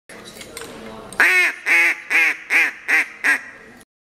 Cabela's calls to some from new Avon, OH, store
Curt, an outfitter at Cabela's, demonstrate a duck call, one of several he carries to demonstrate wares.